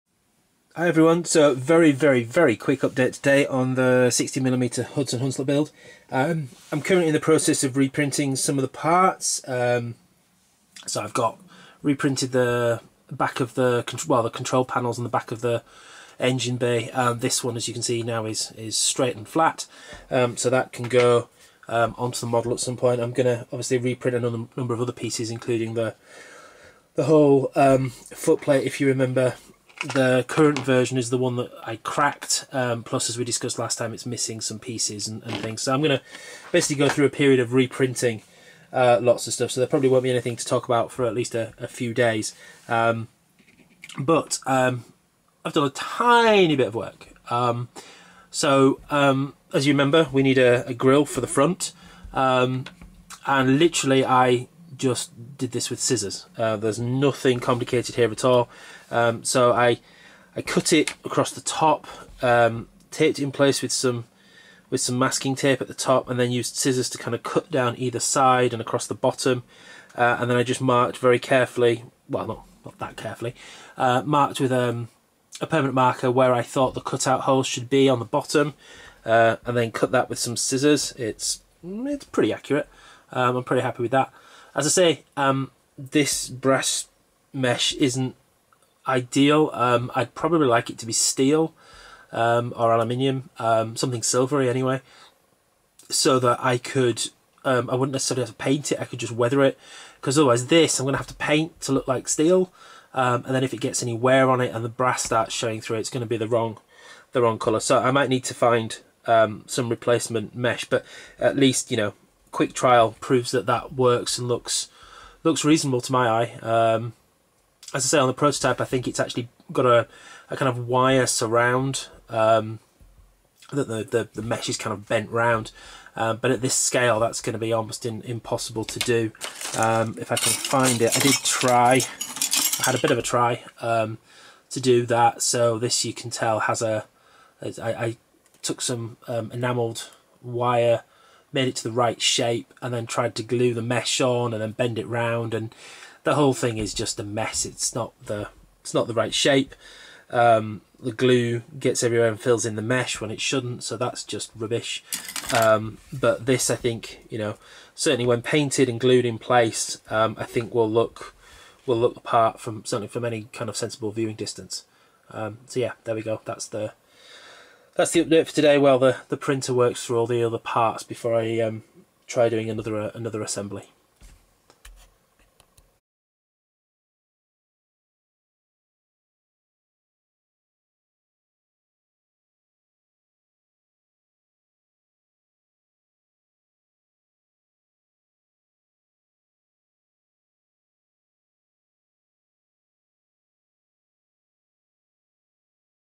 Building a 24hp Hudson Hunslet Diesel Locomotive in 16mm Scale: Front Grill
I'm currently re-printing most of the parts (to deal with known problems etc.) so there will be a bit of a delay before I can make much more progress on assembling a model but here is short update on the metal grill for the locomotives front.